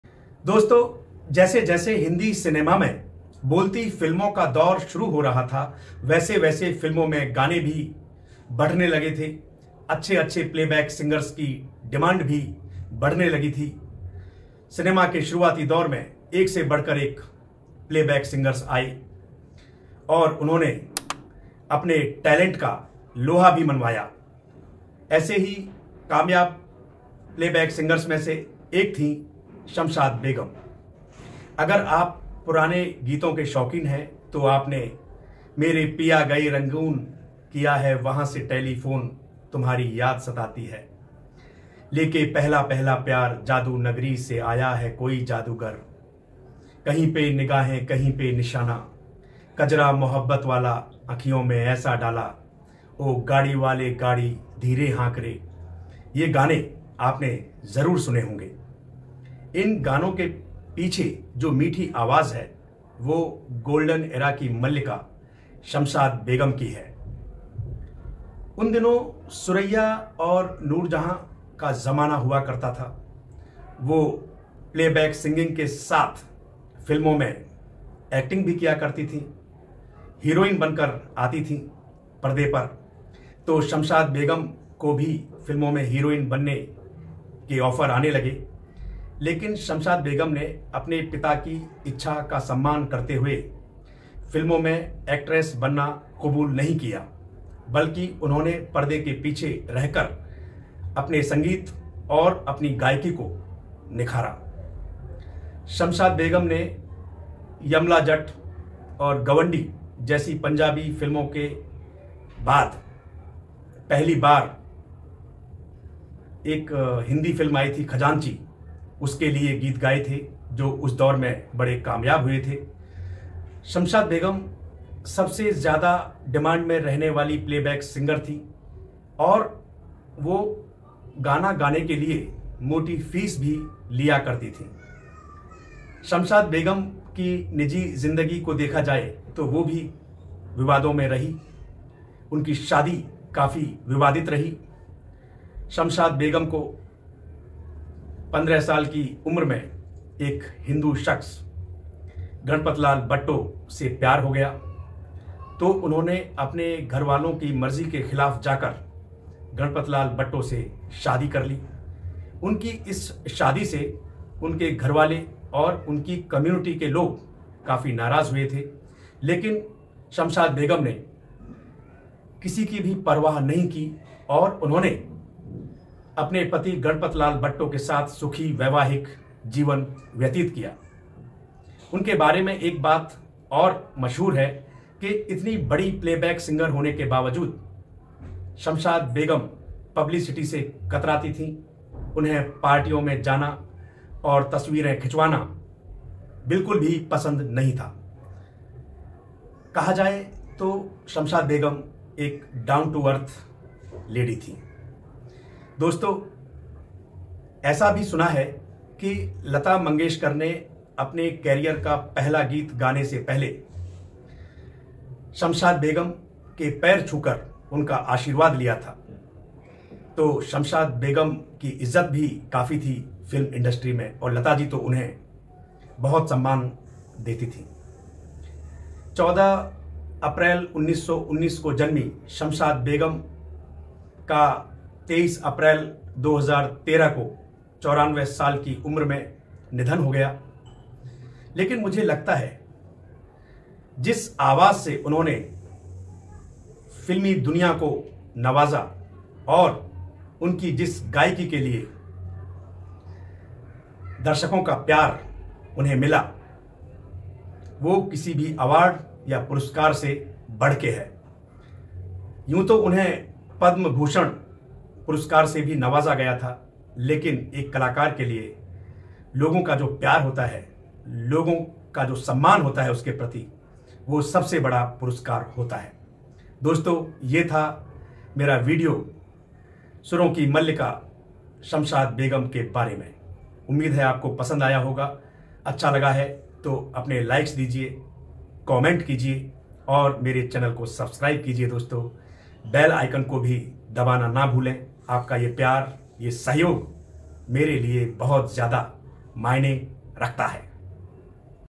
सबसे ज़्यादा डिमांड में रहने वाली golden era की मल्लिका ! Shamshad Begum | Rajesh Manthan | Latest
A video about melodious playback singer Shamshad begum.

दोस्तो ! लीजेंडरी शमशाद बेगम भारतीय सिनेमा के स्वर्णिम युग की एक लोकप्रिय और सदाबहार पार्श्व गायिका थीं। उस ज़माने में उनके गाए गीत फ़िल्म के चलने की गारंटी हुआ करते थे, और वो अपने गीतों के लिए मोटी फ़ीस भी लेती थीं। उनकी ज़िन्दगी भी उन दिनों काफ़ी सुर्खियों में रहा करती थीं। उनकी शादी भी उन दिनों काफ़ी विवादित रही थी। तो आइए जानते हैं सुरों की मल्लिका शमशाद बेगम से जुड़े कुछ दिलचस्प अनसुने क़िस्से...

Research & Narration : Rajesh Manthan